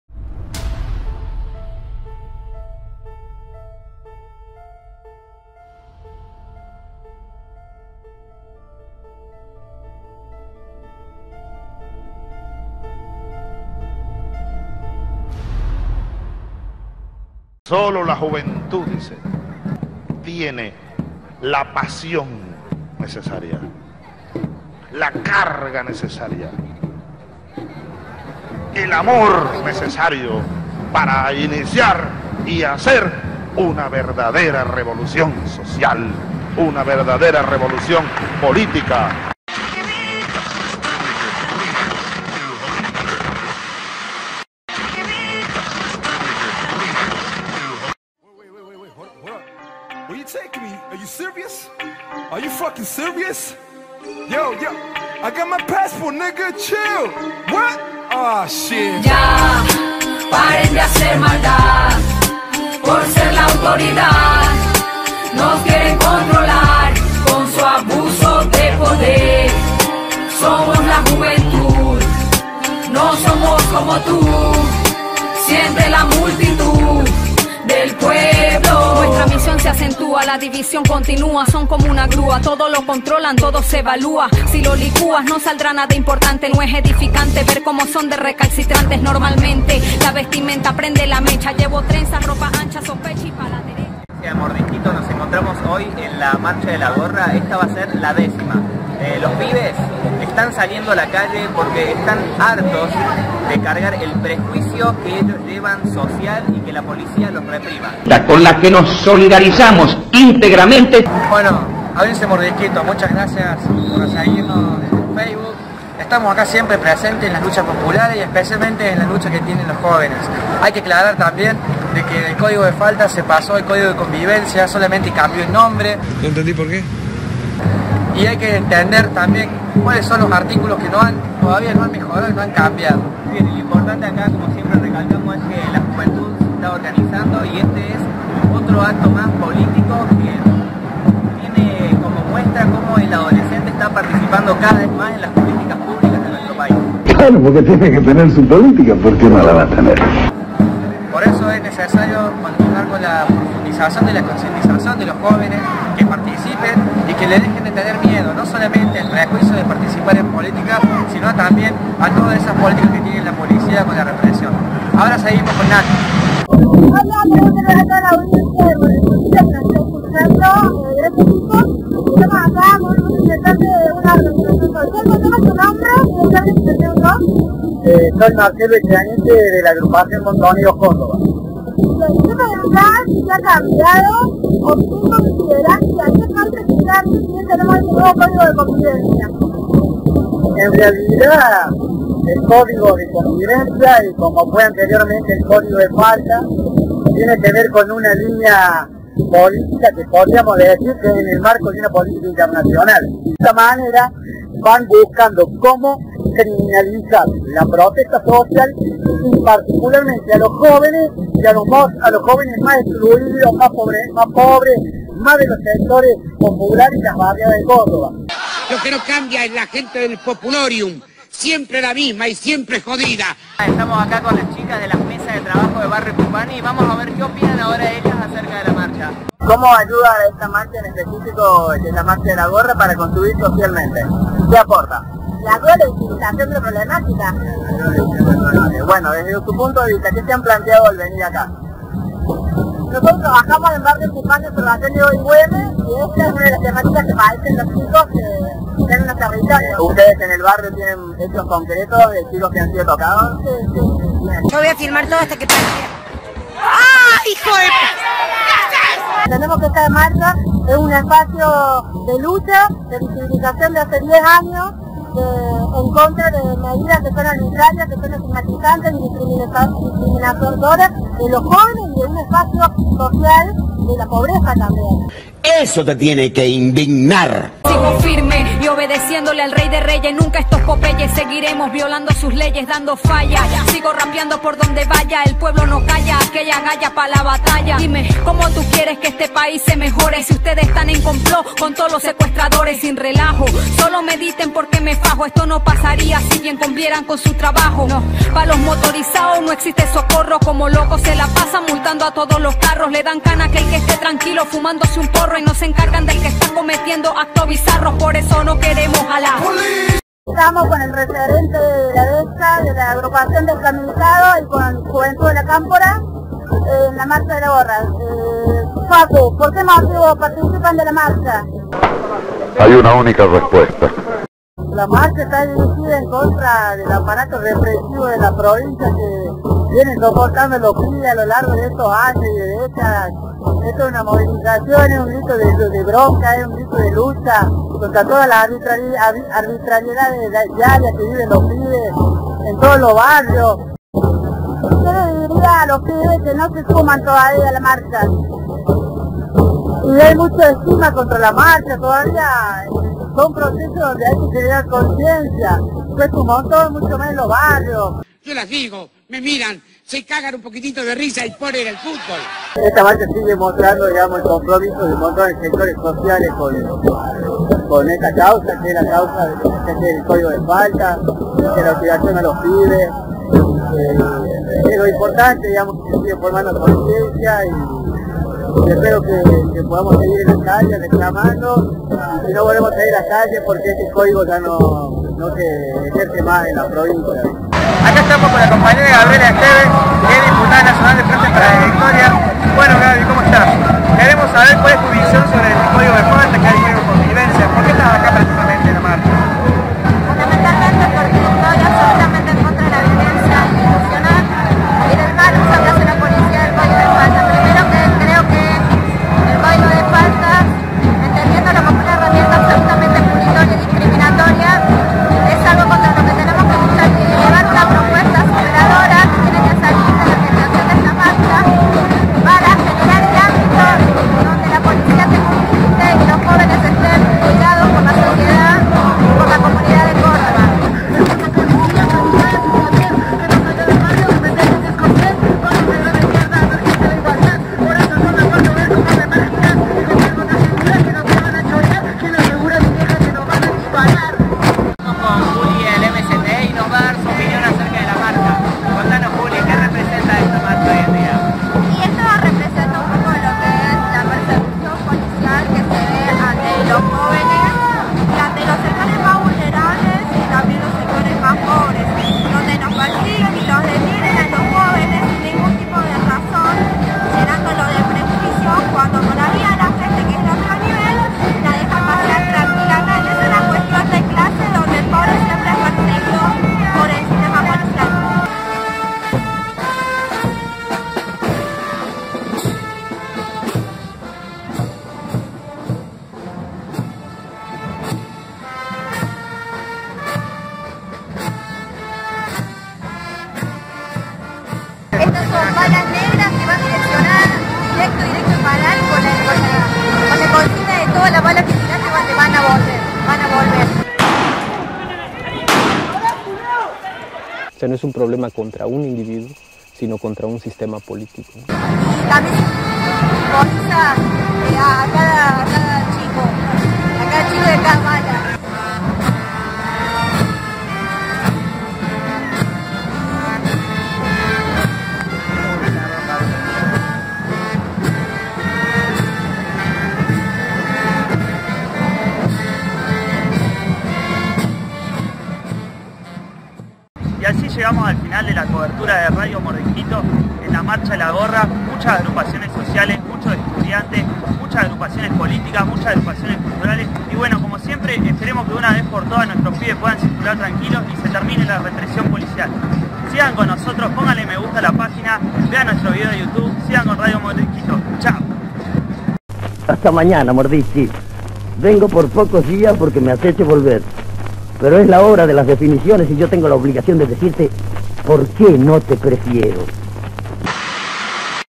Marcha de La Gorra
Aqui le dejamos a todos ustedes , la cobertura completa de la Marcha de la Gorra que ocurrió el pasado viernes 18 de Noviembre. Se lo prometimos , se lo cumplimos. Mordisquito , decimos lo que otros no se animan a decir.